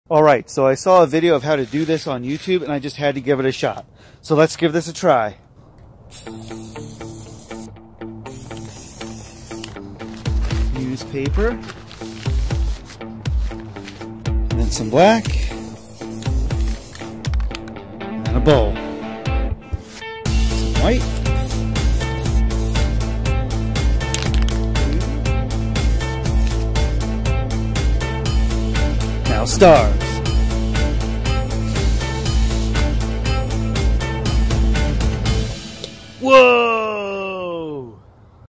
YouTube Short - Making A Planet!
Watch me make a planet - using spray paint!